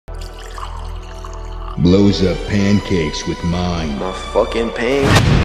Pancakes
Za'riel enjoying his pancakes until Yonni..